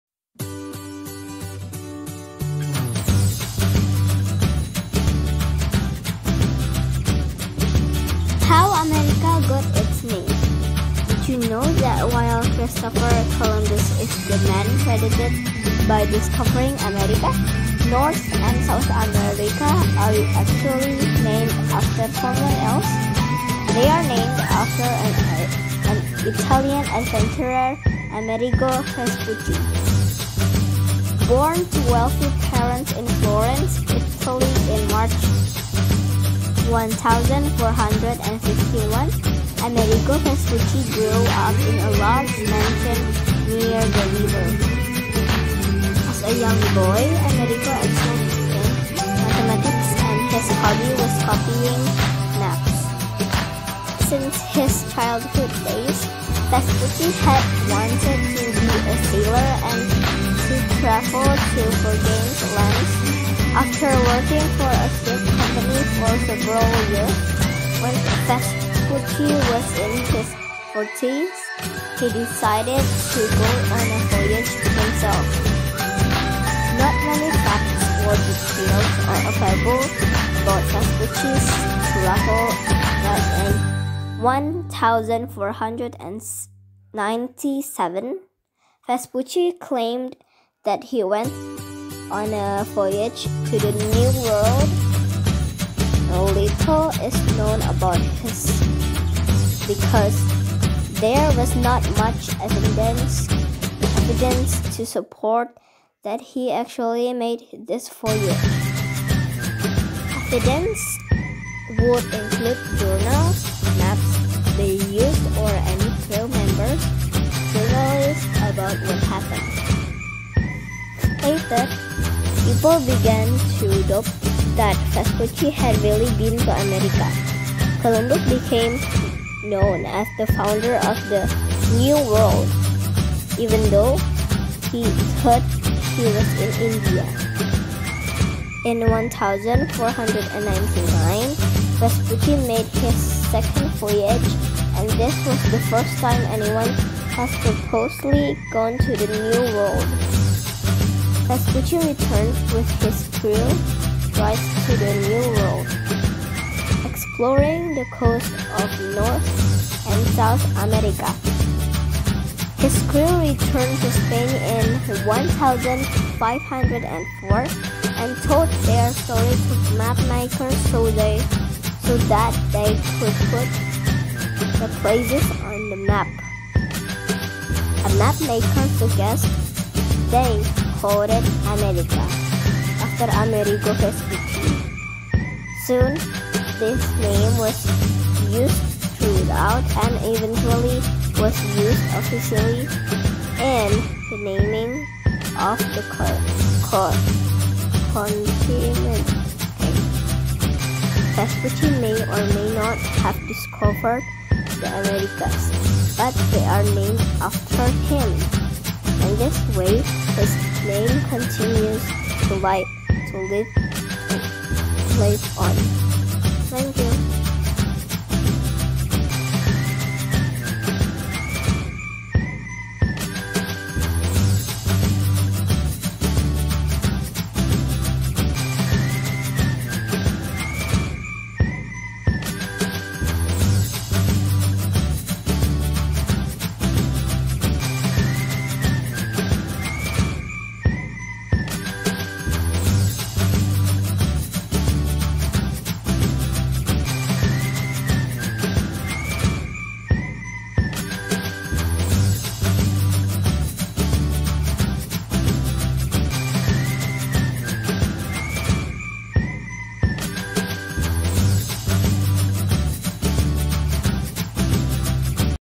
How America got its name
Amerigo Vespucc, Italian: [ameˈriːɡo veˈsputtʃi]; 9 March 1454 – 22 February 1512) was an Italian-born merchant, explorer, and navigator from the Republic of Florence, from whose name the term "America" is derived. He became a Castillian citizen in 1505.